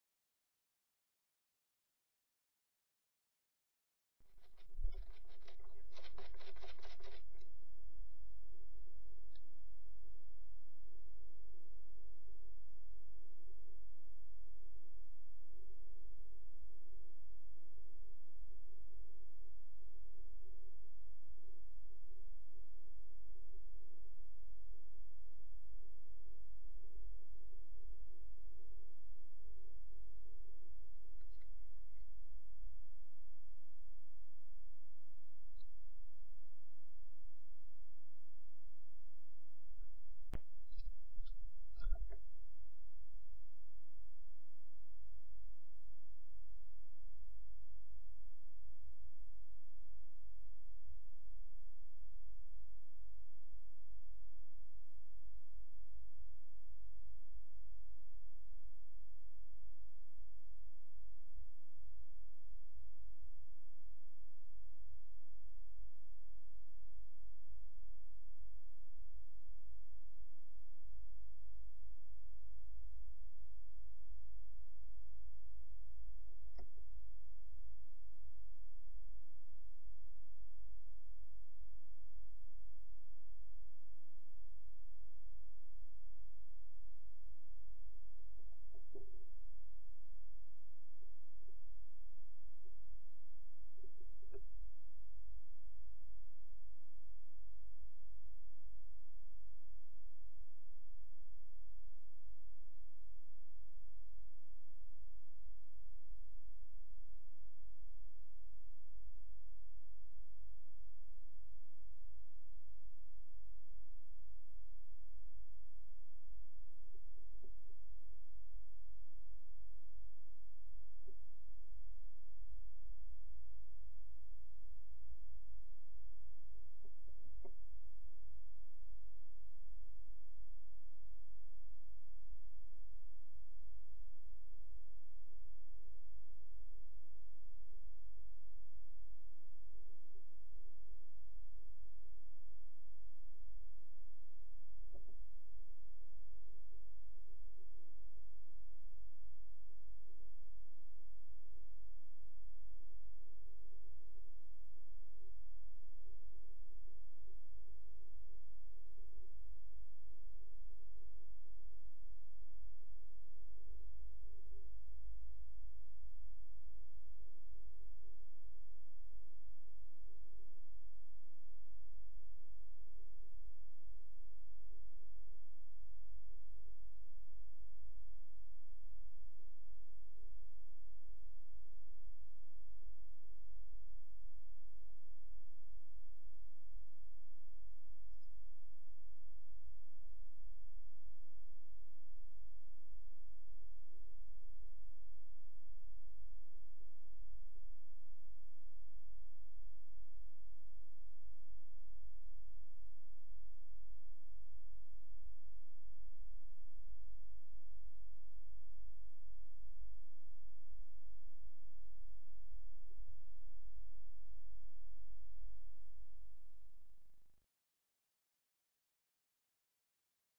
02FEB17 Steenuil2 lekkerbuiten
Het zonnetje schijnt, droog en windstil, dus de steenuiltjes zitten lekker buiten. En die camera is ook wel interessant...